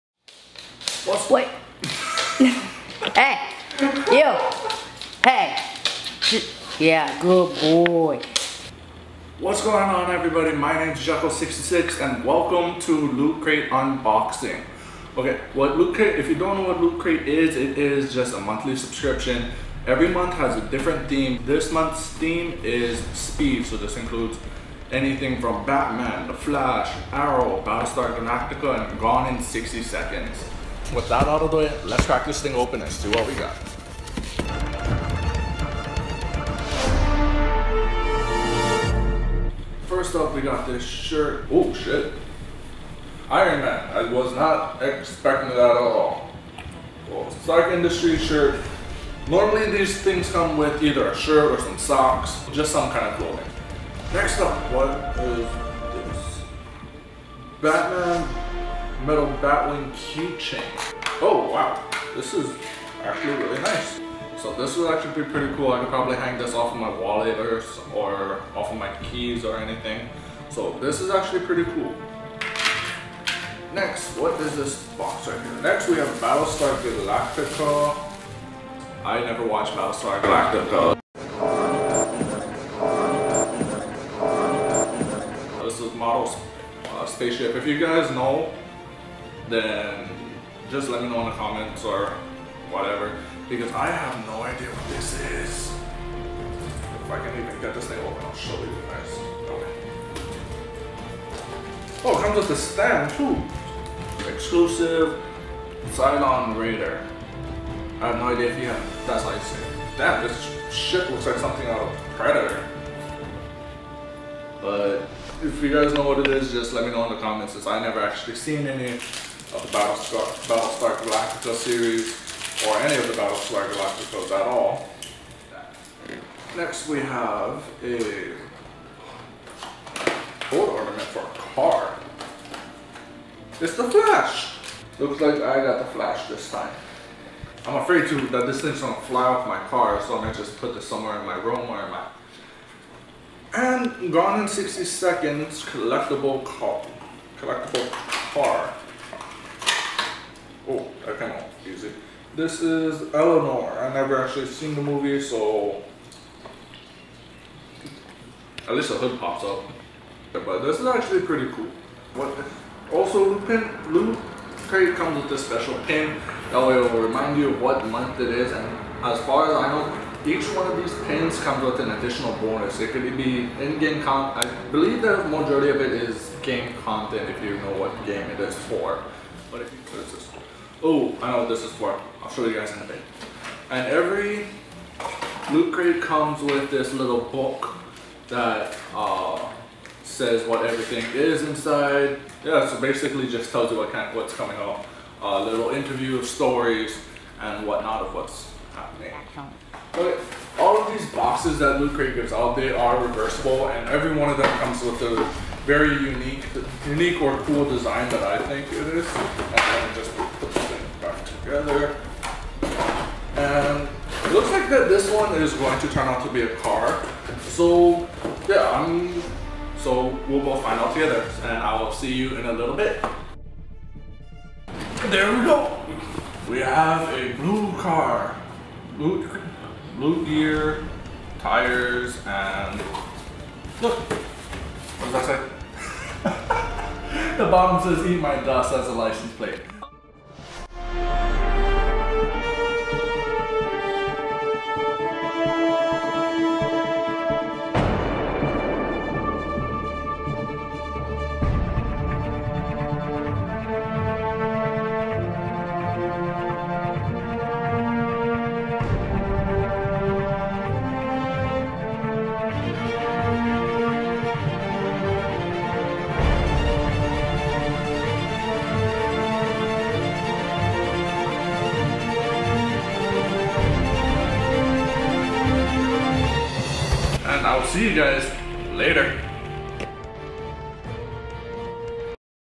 September 2016 LootCrate Unboxing
What's going on guys! It is finally finished. Due to some difficulties with getting together with my editor we were unable to release this video in September but we finally did it.